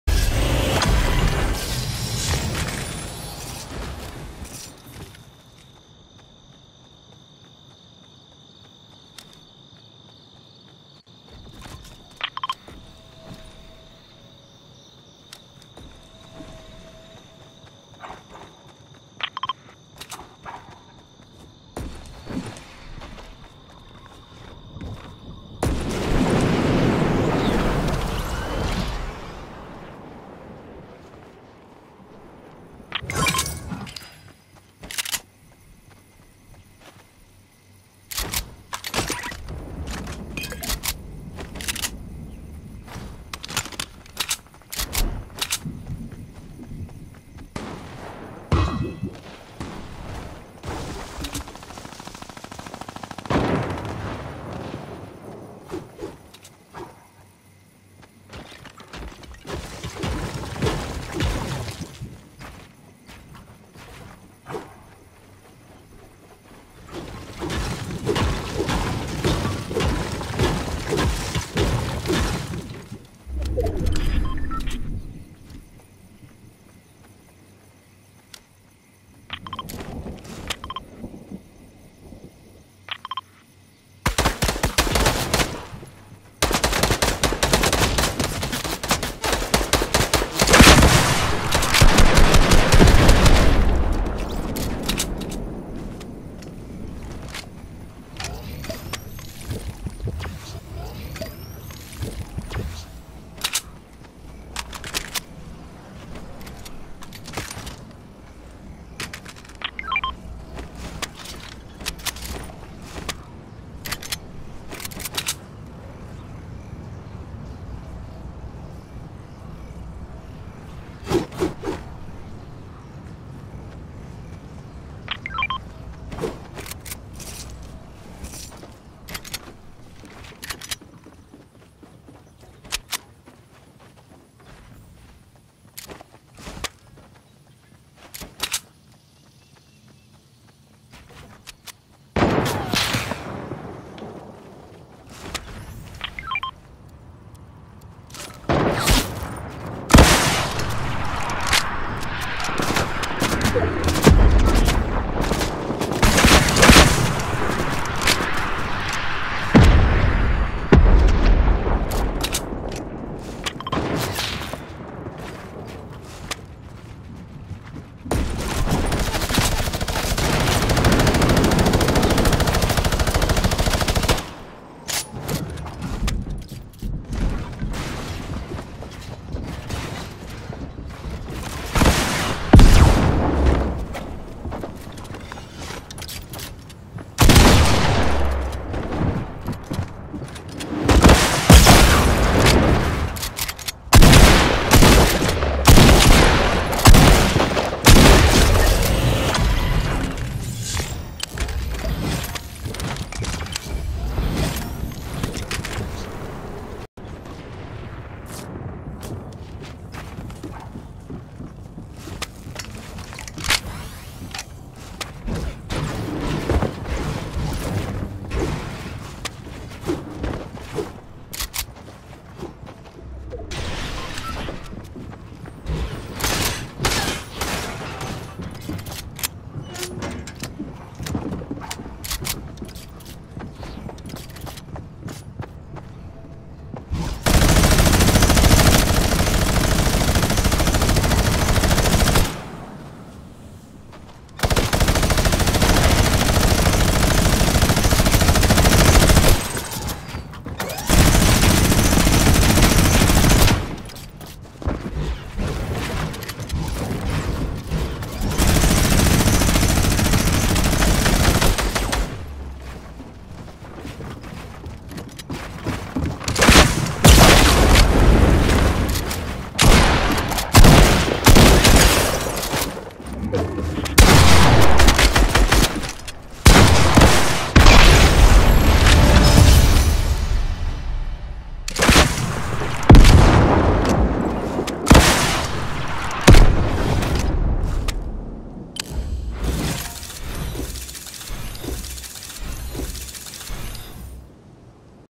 ¡Victoria! | Las Fuerzas Especiales × ElWider092
"Nombe vato, mugre partida fea"
- ElWider092

Esta partita estuvo bien rara, ElWider092 y Las Fuerzas Especiales calleron en "La Plataforma" (suroeste del mapa), se presentaron problemas en plena tormenta que nos alcanzo, para después caminar 1400mts hasta la "zona"  con poca vida y poca munición. Poco a poco se nos fueron favoreciendo las cosas para así poder llegar a la victoria.

Vayan y siganlo en su perfil de instagram, sube buenos clips.